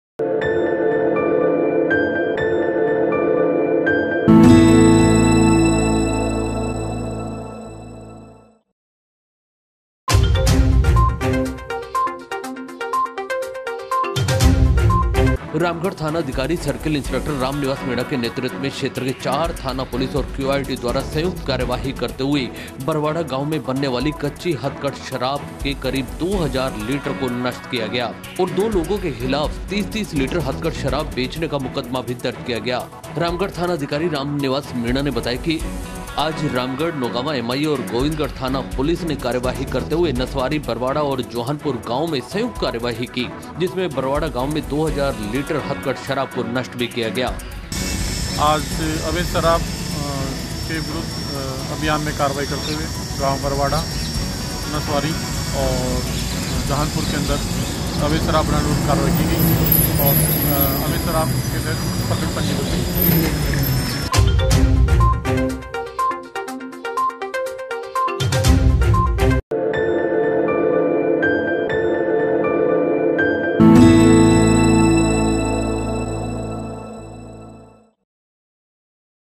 अवैध शराब के खिलाफ रामगढ़ पुलिस की बड़ी कार्यवाही ,
अवैध शराब के खिलाफ रामगढ़ पुलिस की बड़ी कार्यवाही , गोविंदगढ़ ,नोगांवा व एमआईए थाना पुलिस व क्यूआरटी ने की कार्यवाही ।